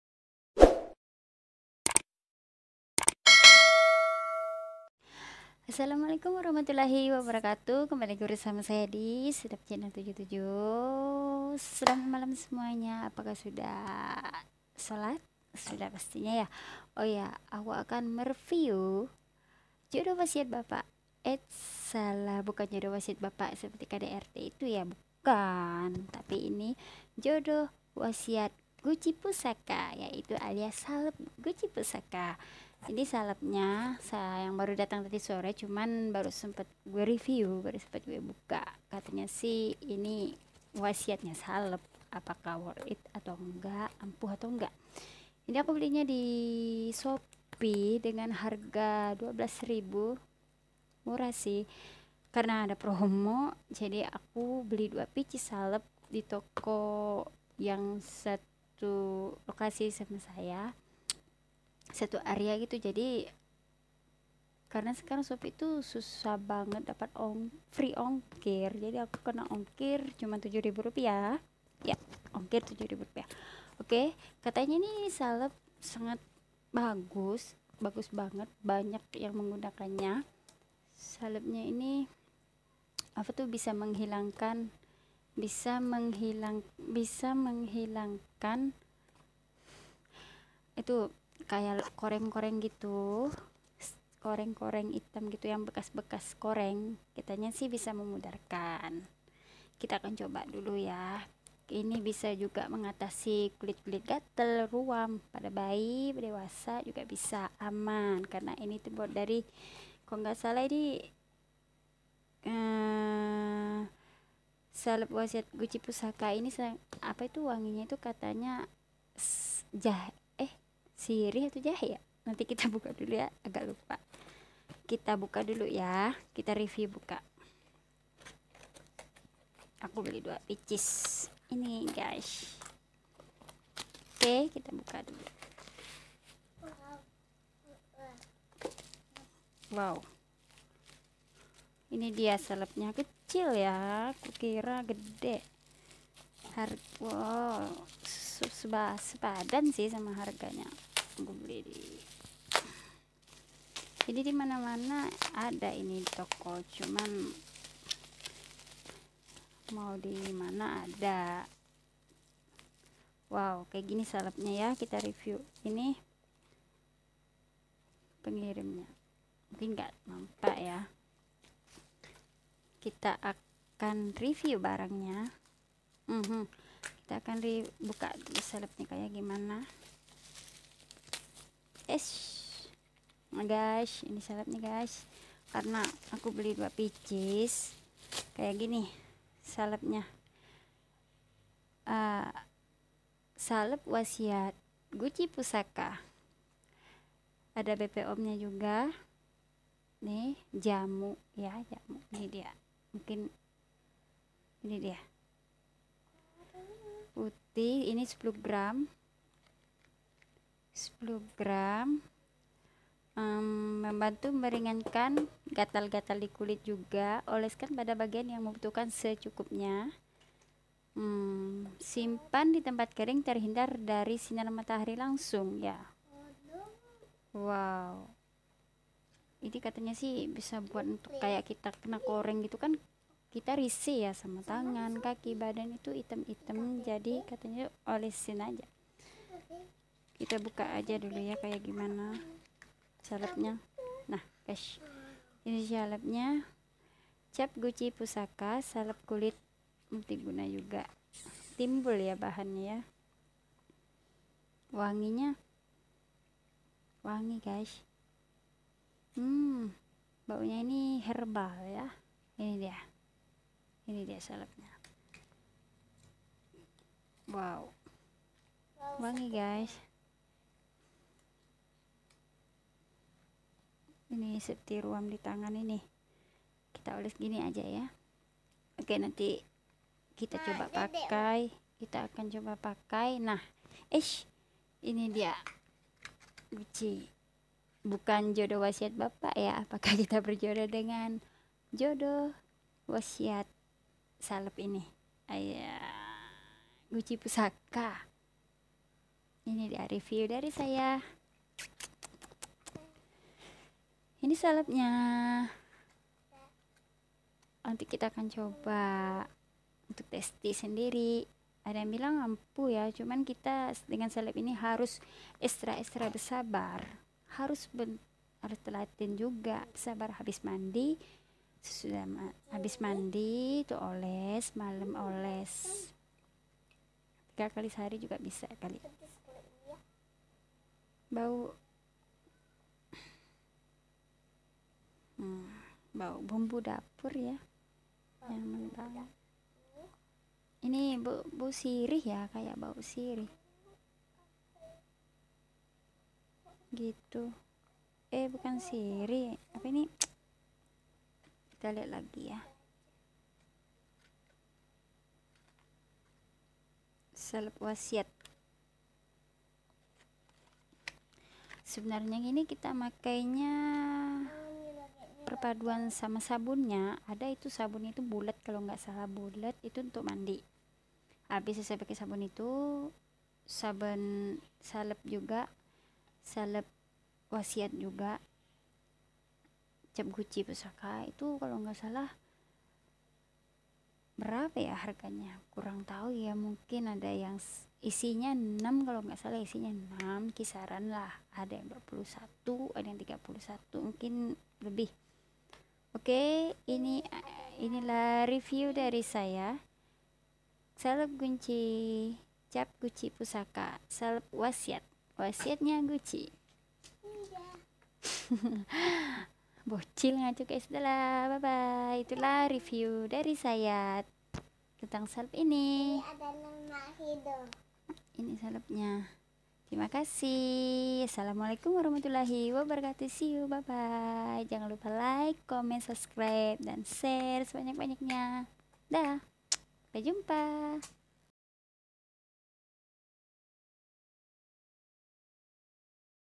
salep wasiat guci pusaka ampuh semua jenis kulit#mengatasibekaskoreng#gatalkulit#review#unboxing
assalamualaikum warahmatullahi apa kabar semua,saya mau ngeri view salep kulit ampuh mengatasi gatal-gatal menghilangkan bekas koreng juga kemerahan